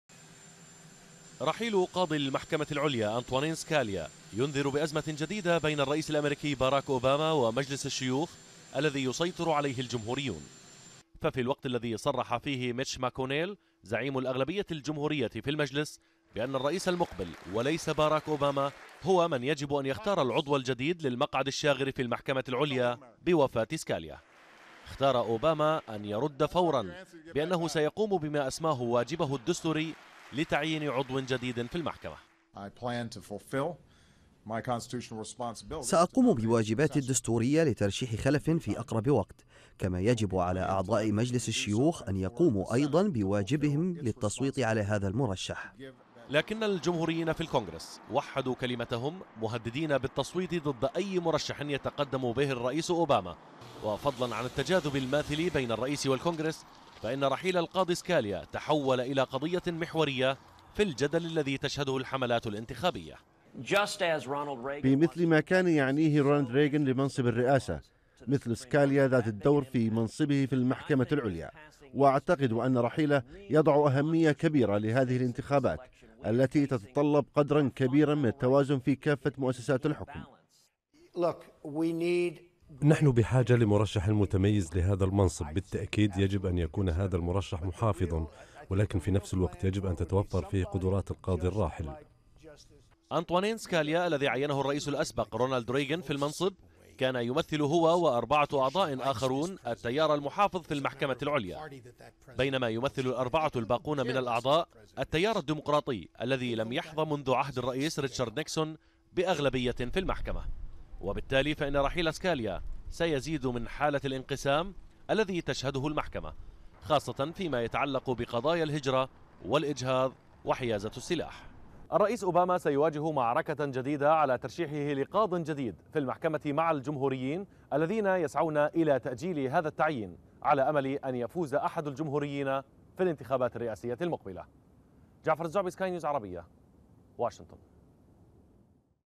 الجمهوريون يسعون لعرقلة قرار أوباما بتعيين خلف لسكاليا
دعا المرشحون لنيل ترشيح الحزب الجمهوري إلى الانتخابات الرئاسية الأميركية مجلس الشيوخ لعرقلة تعيين أي قاض يختاره الرئيس باراك أوباما لخلافة عضو المحكمة العليا المحافظ أنطوانين سكاليا الذي رحل يوم السبت الماضي.